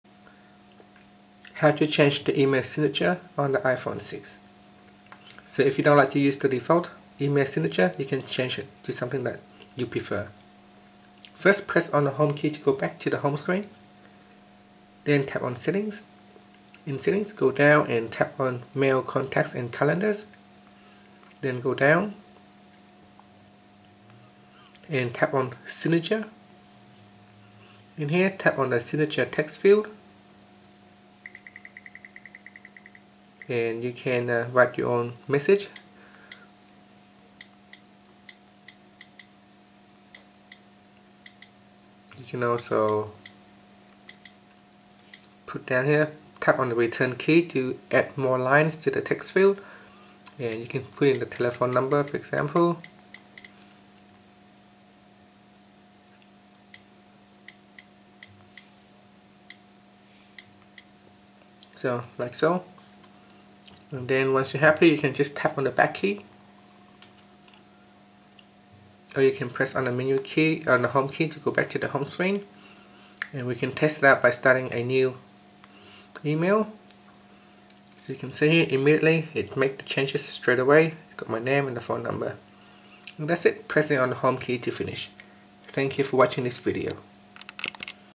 iPhone 6: How to Add / Change Email Signature
Learn how you can add or change the email signature on the iPhone 6.